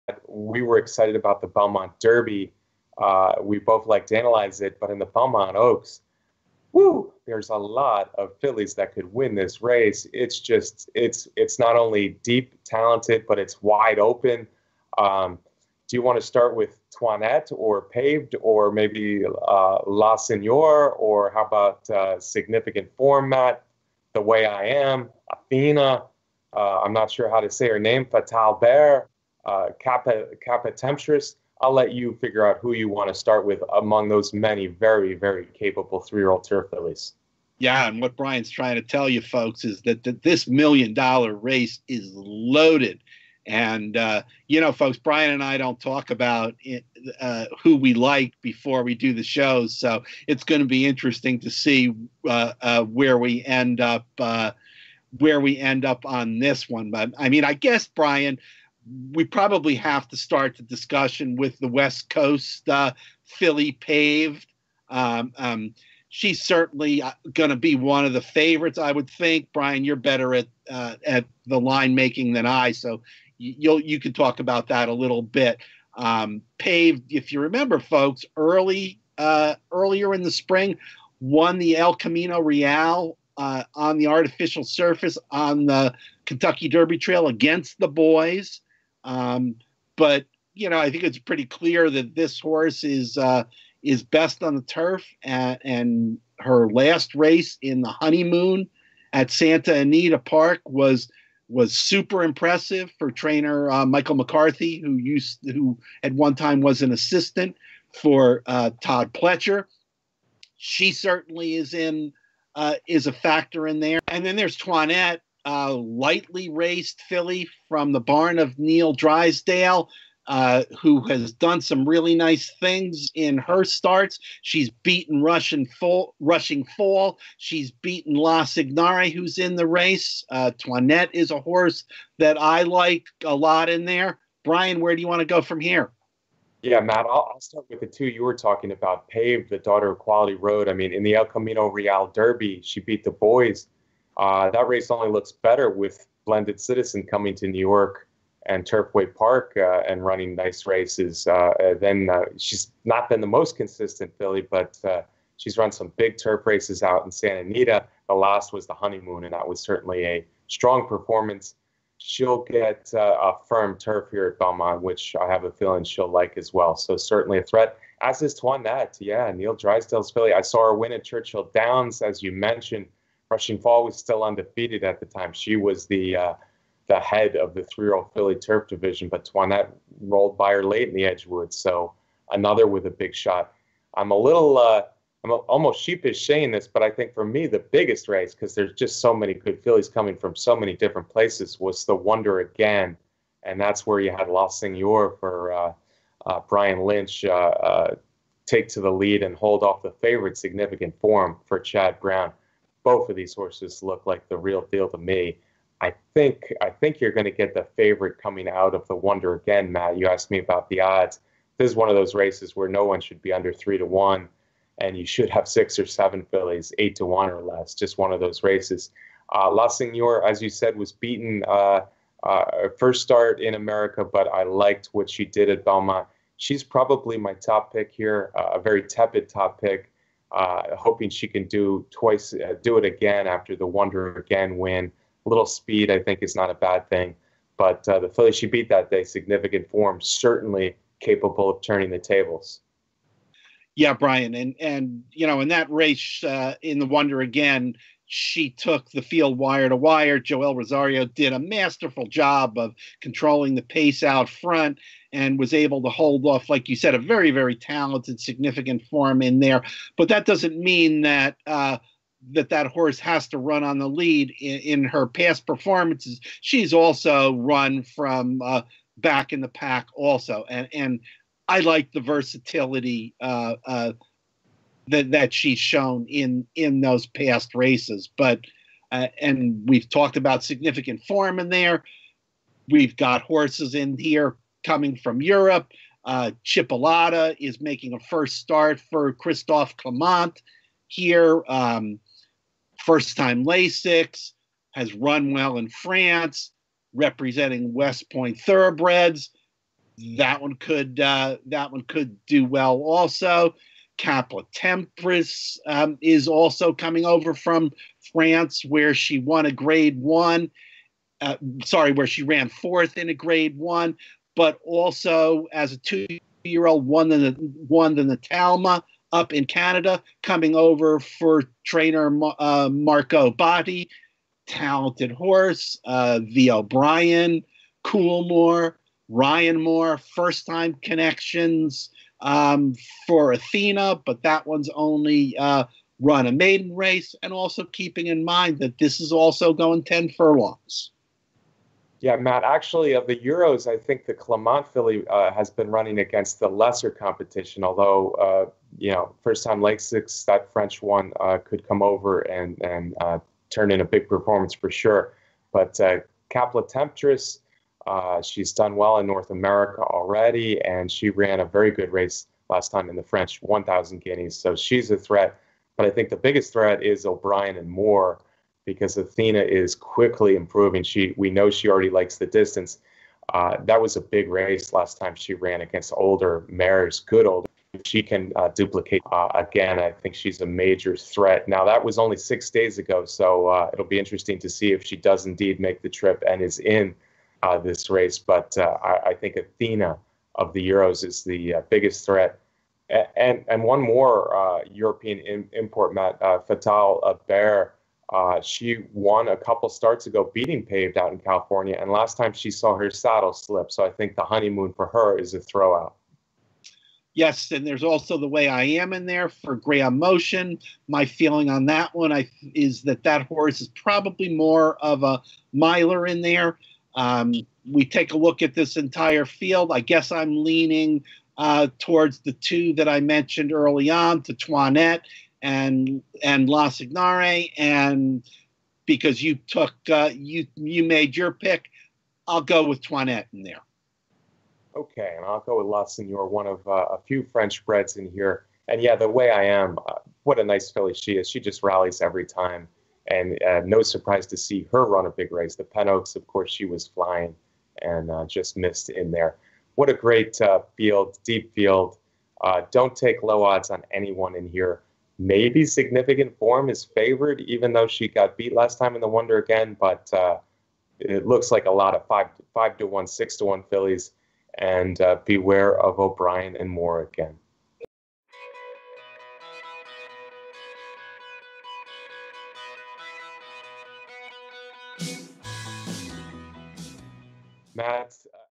Belmont Oaks 2018 picks and predictions
Seven graded stakes winners converge in Saturday's Belmont Oaks Invitational on the Stars & Stripes card in New York. HorseCenter hosts Matt Shifman and Brian Zipse break down the race.